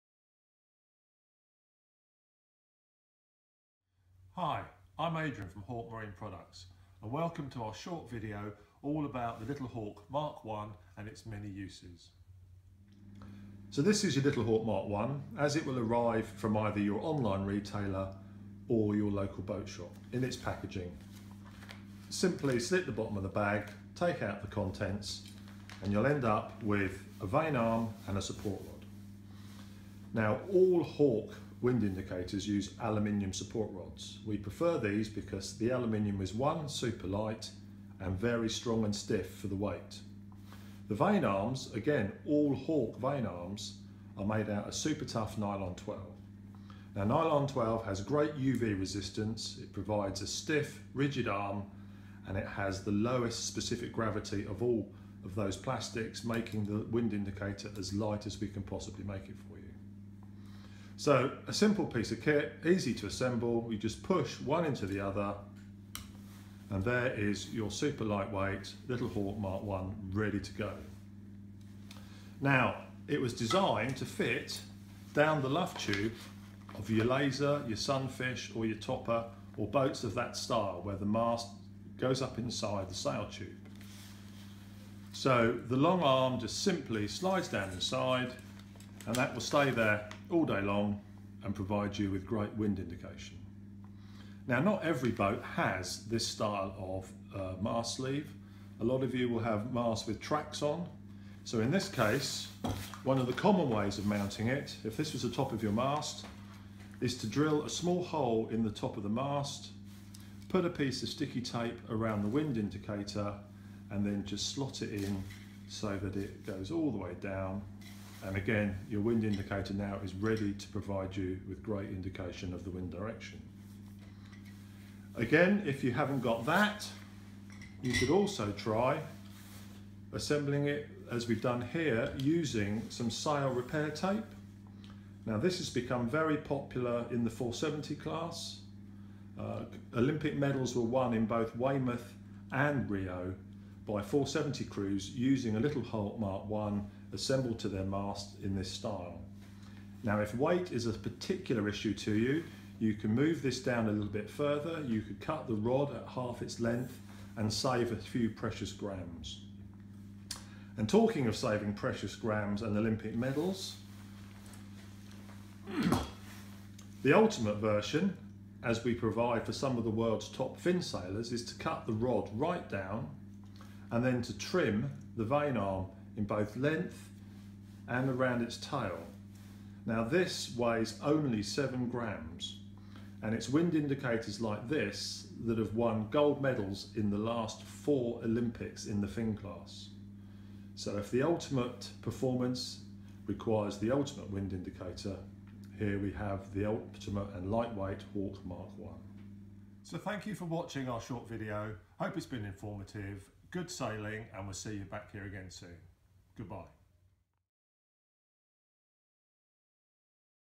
Little Hawk MK1
A Close up look at the Little Hawk MK1 from Hawk Marine Products.